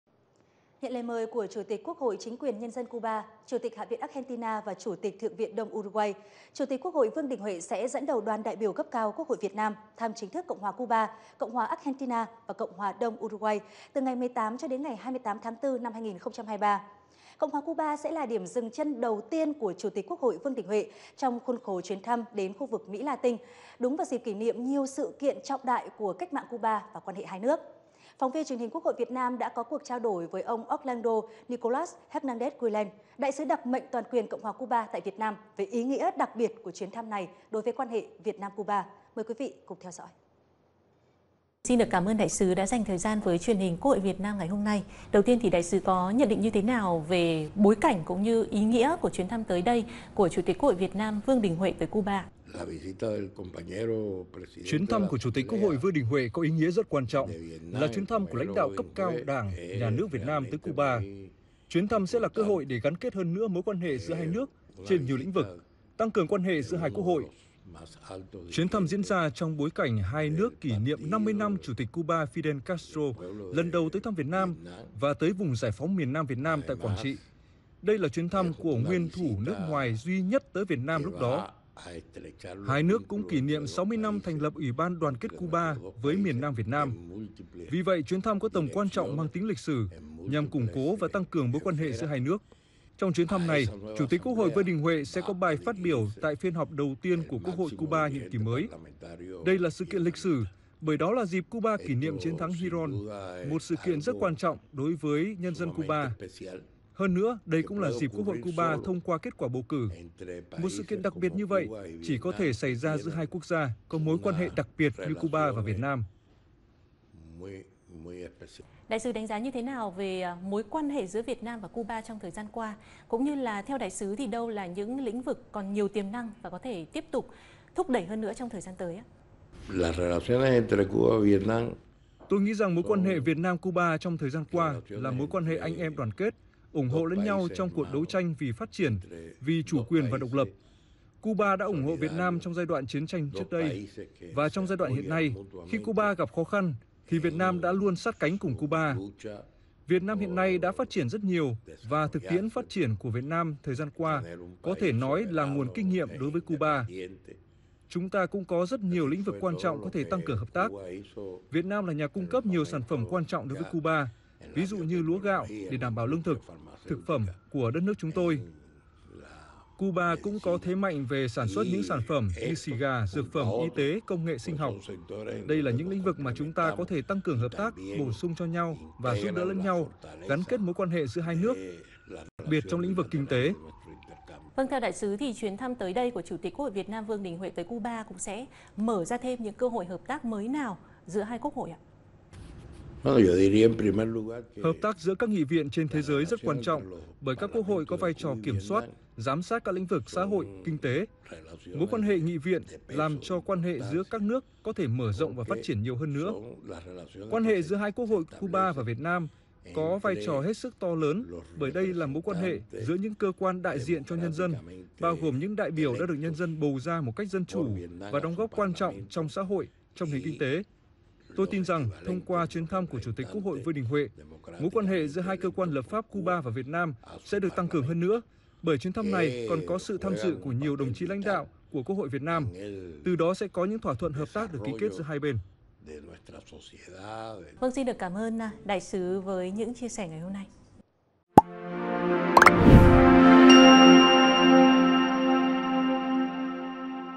Đại sứ Cuba: Chuyến thăm của Chủ tịch Quốc hội Vương Đình Huệ có ý nghĩa rất quan trọng
Truyền hình Quốc Hội Việt Nam | Chủ tịch Quốc hội Vương Đình Huệ sẽ thăm chính thức Cuba từ ngày 18-28/4. Chuyến thăm nhằm tái khẳng định Việt Nam luôn coi trọng tiếp tục củng cố, tăng cường quan hệ đoàn kết, hữu nghị đặc biệt, mẫu mực thủy chung giữa hai Đảng, hai nước.